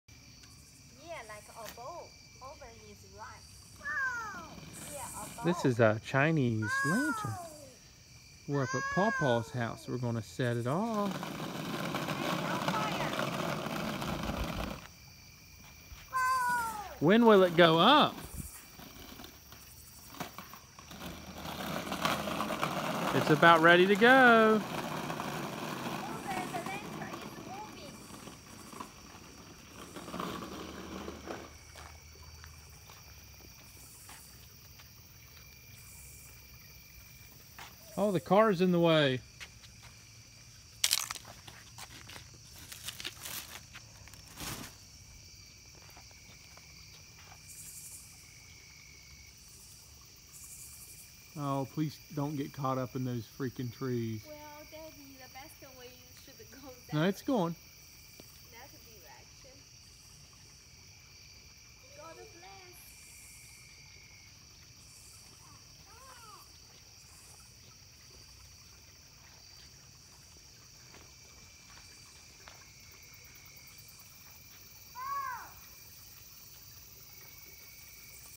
how to set of a chinese lantern
i love watching these go up and away.  like the video if you do too